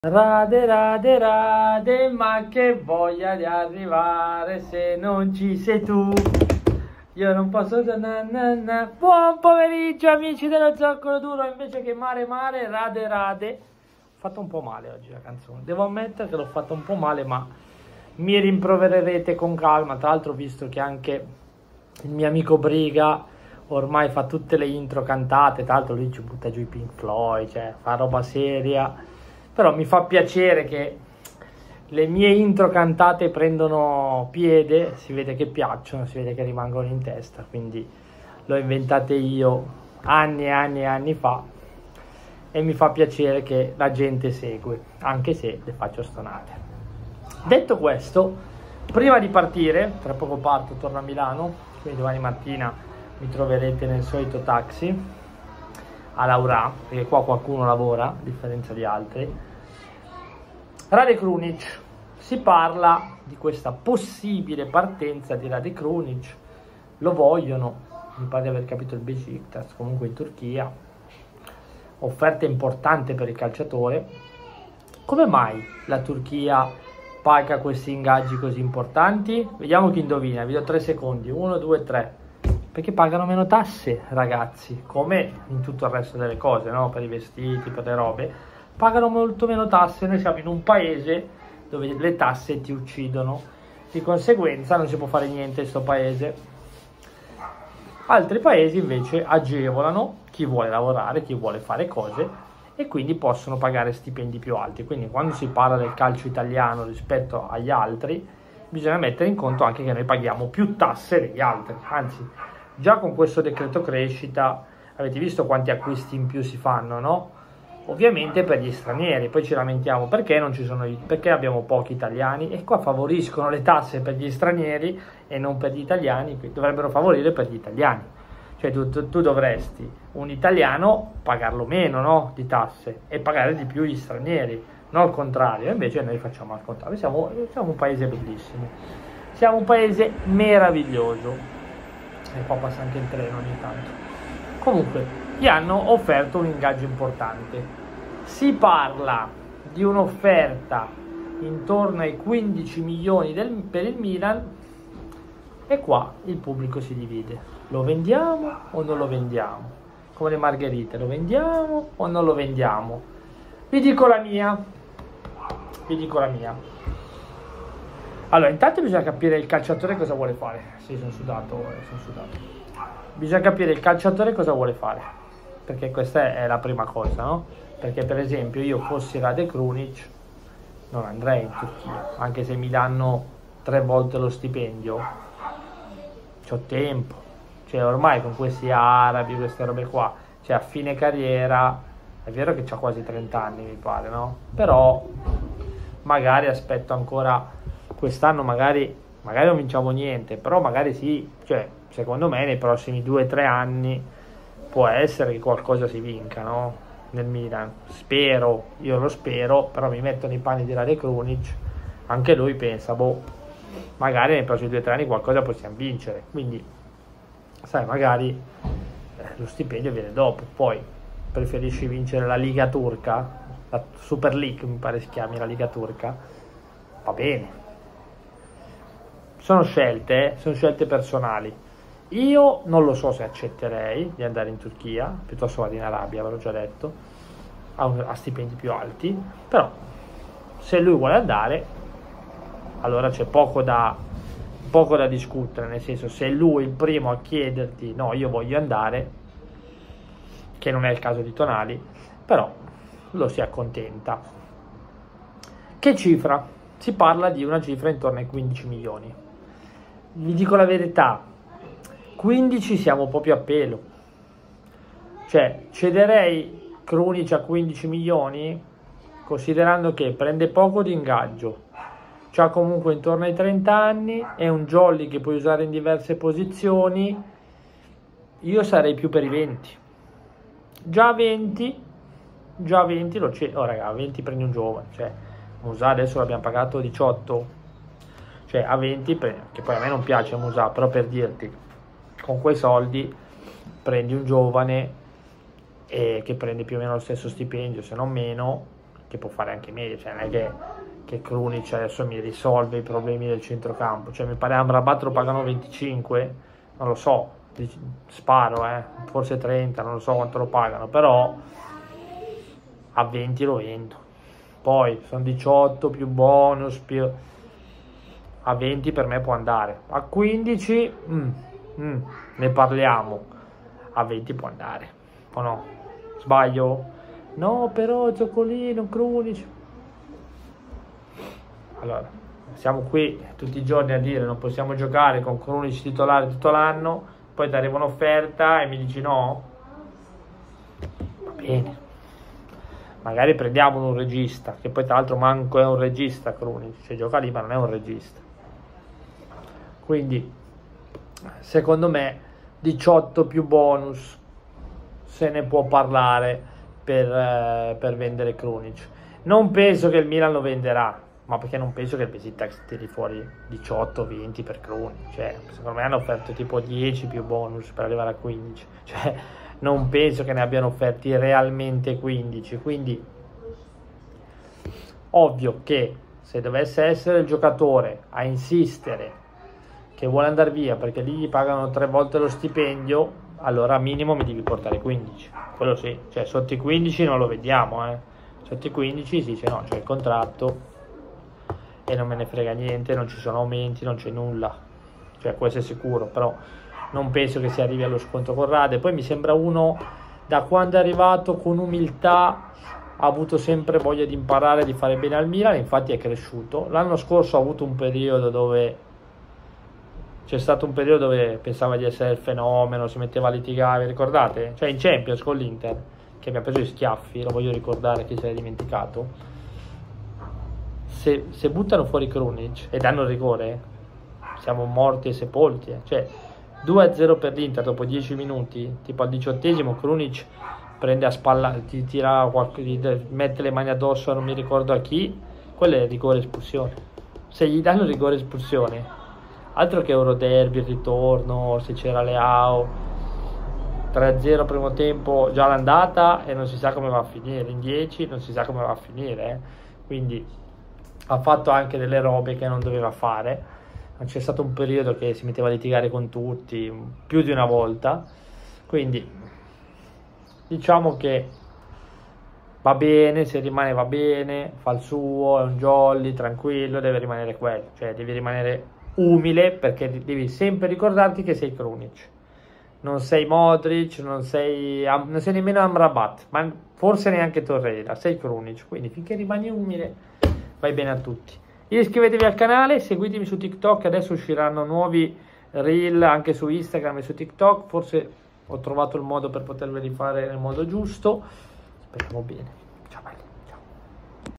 KRUNIC: VENDERE O NON VENDERE?
Parliamo di questa fantomatica offerta per Krunic..

REGISTRATI SU SATISPAY!!

INSTAGRAM PERSONALE:
Luca.magno.life